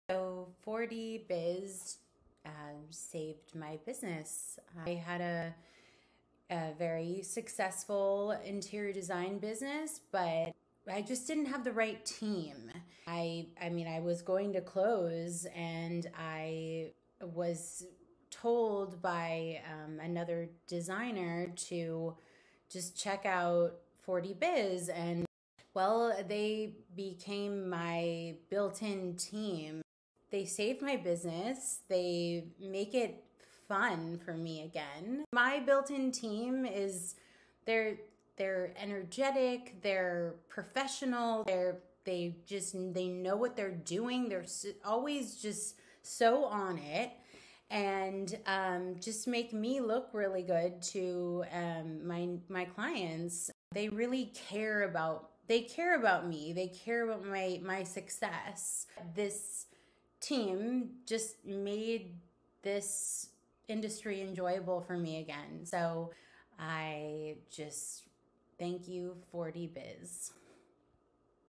Hannah Lowe Interiors + 4Dbiz
Before working with 4Dbiz, Hannah was burned out, stressed, and nearly ready to close her interior design business for good. Now, she has a built-in team  that helps her manage (and actually enjoy!) her work again.